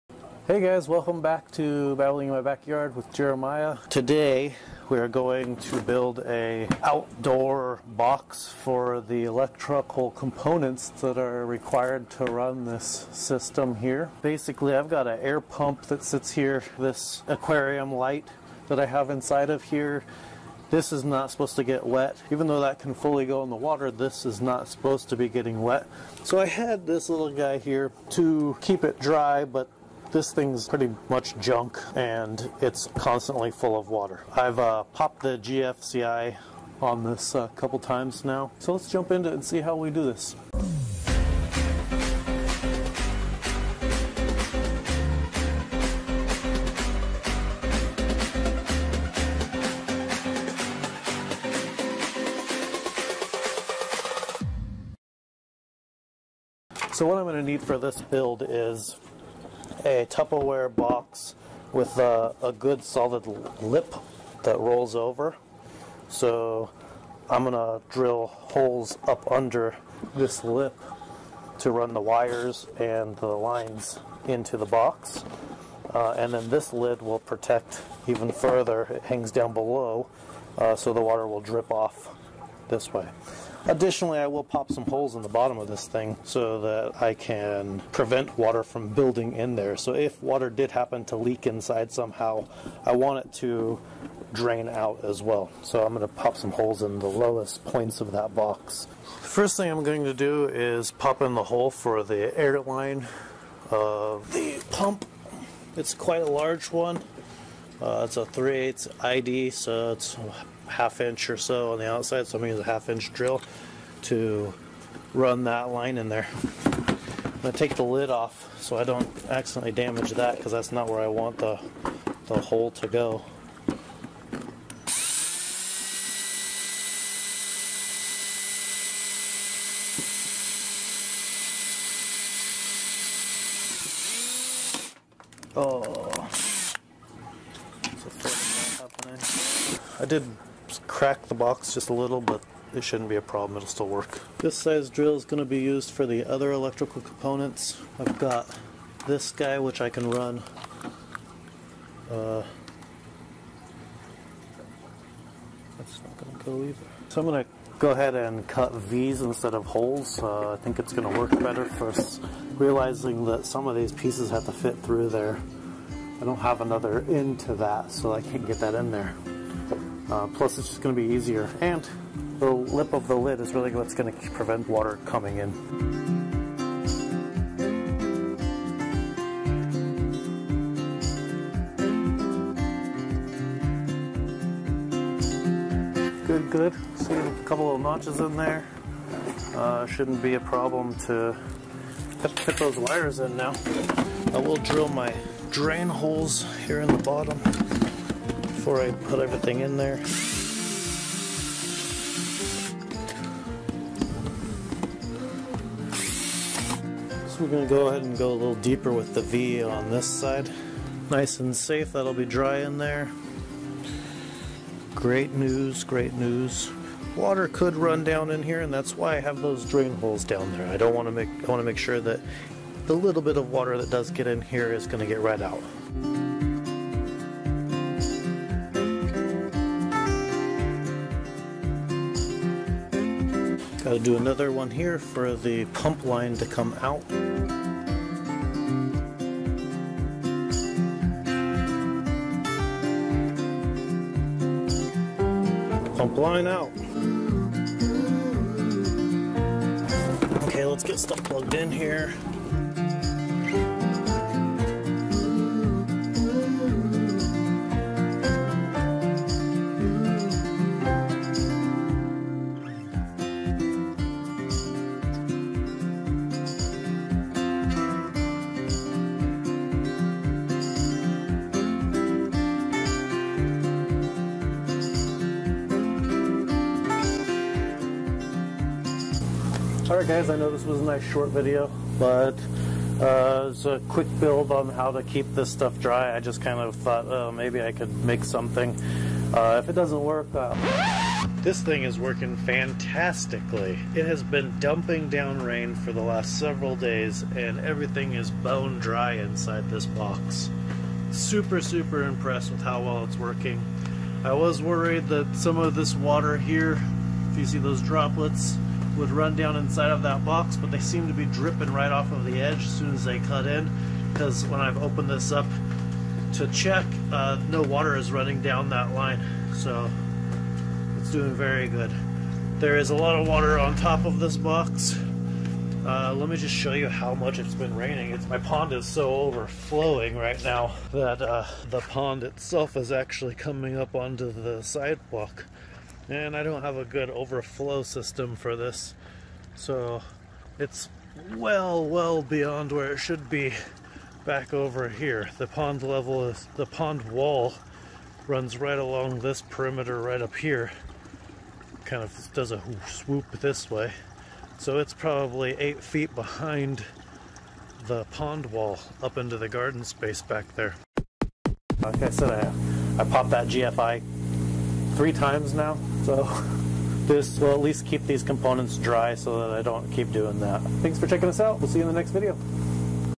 DIY Waterproof Outdoor Electrical Box
In this video, I will walk you through how to build an outdoor waterproof electrical box.  This is a DIY method to keep electrical components dry and safe from the weather.  Stop popping your GFI or GFCI and keep it dry.